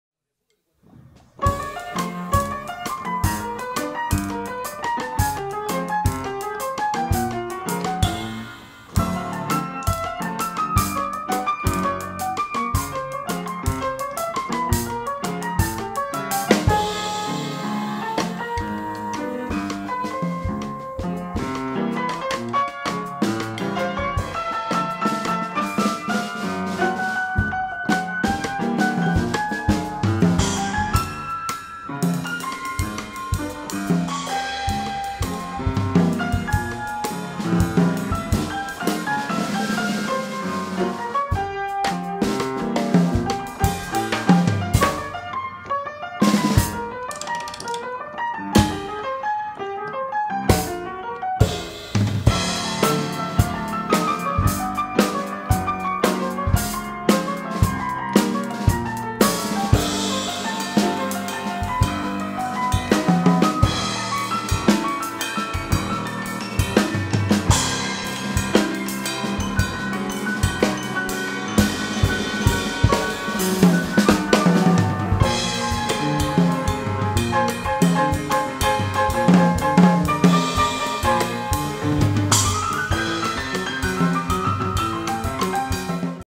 desafiobateril
Desafío Bateríl Fragmento sobre esta gran composición de la genial pianista Marina Ruiz Matta 💪🏽